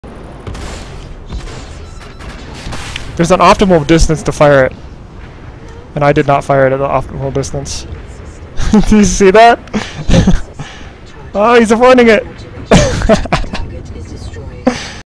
Rattler
Firing the rattler from a cutlass with the new missile indication = fun